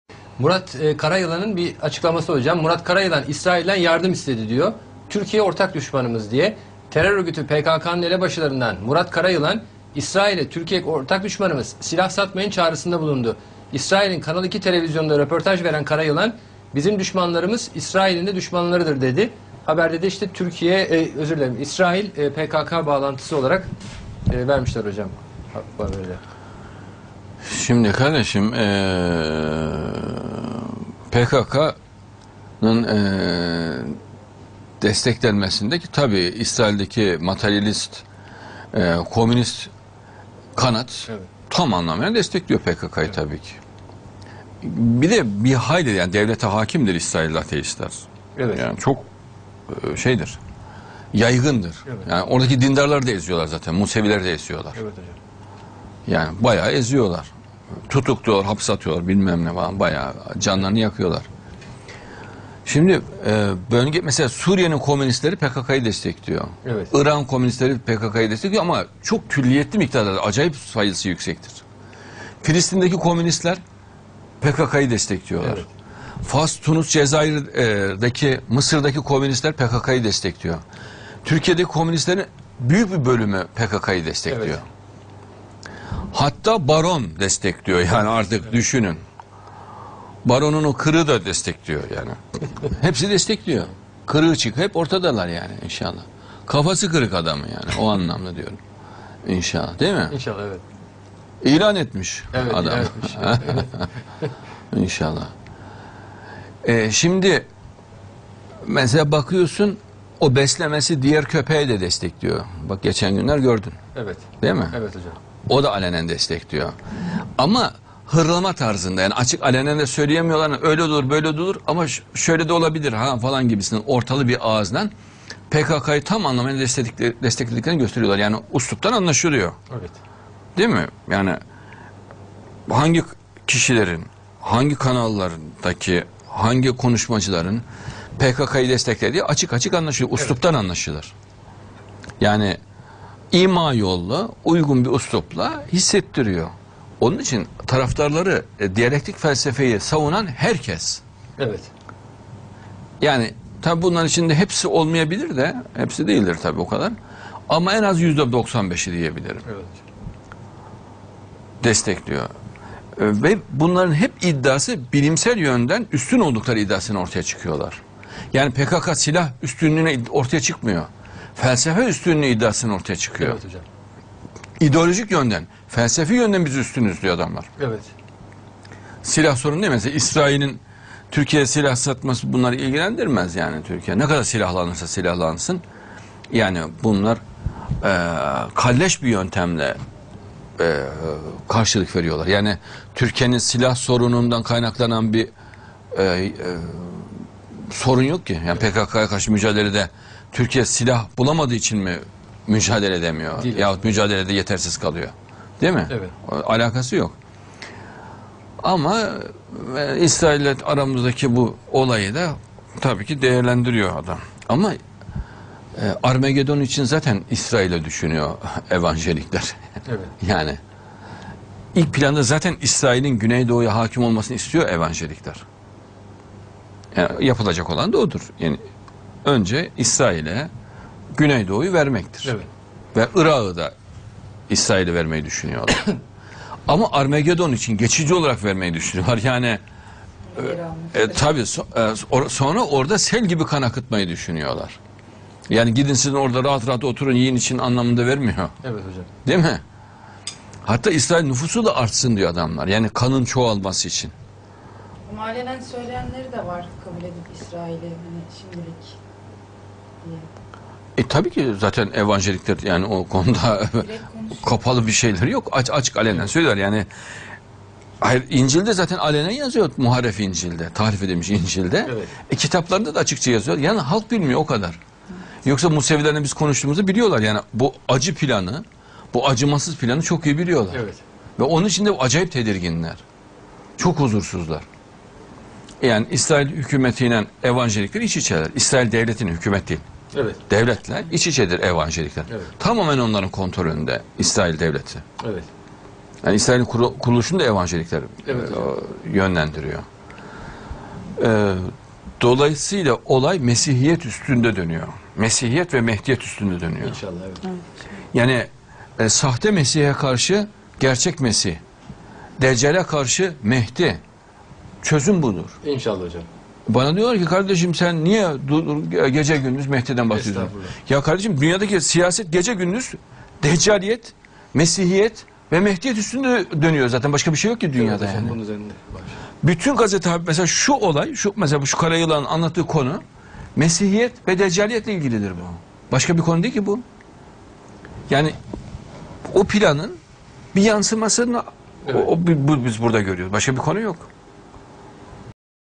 Evanjelikler ilk planda İsrail'in Güneydoğu'ya hakim olmasını istiyor, ama Armagedon...
Evanjelikler ilk planda İsrail'in Güneydoğu'ya hakim olmasını istiyor, ama Armagedon için geçici olarak vermeyi düşünüyor.